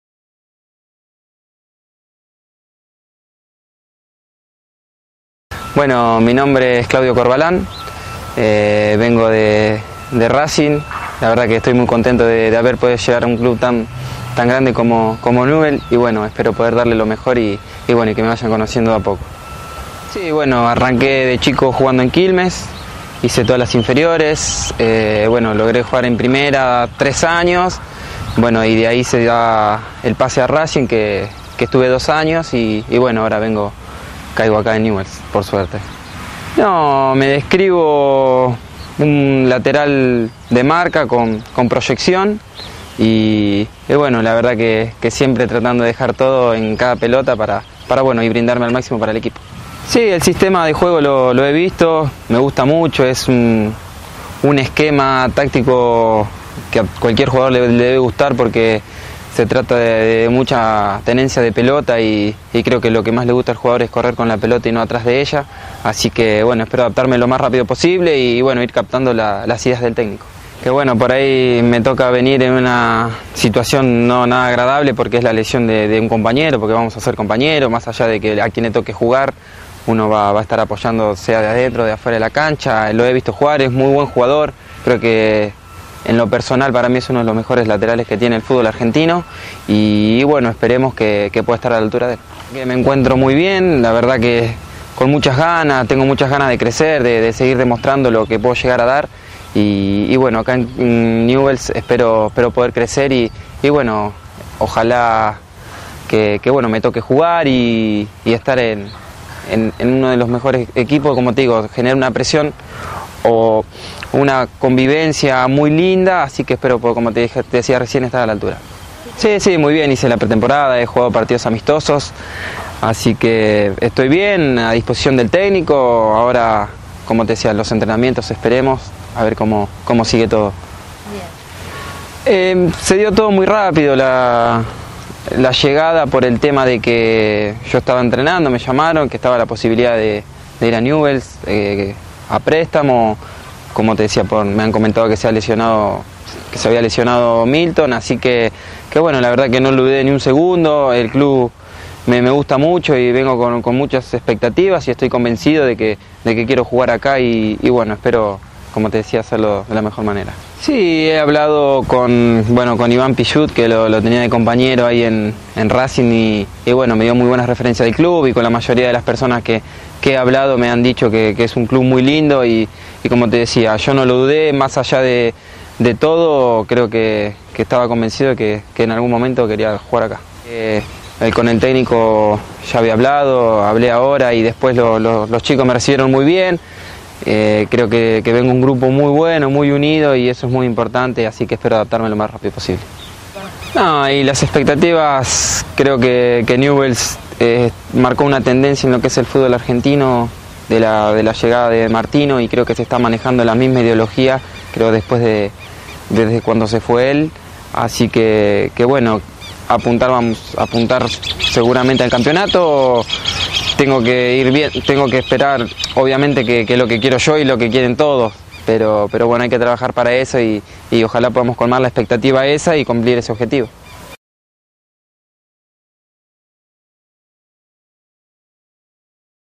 Claudio Corvalán, nuevo jugador de Newell's.
El ex Racing y Quilmes se sumó al plantel dirigido por Gustavo Raggio para el próximo Campeonato 2014.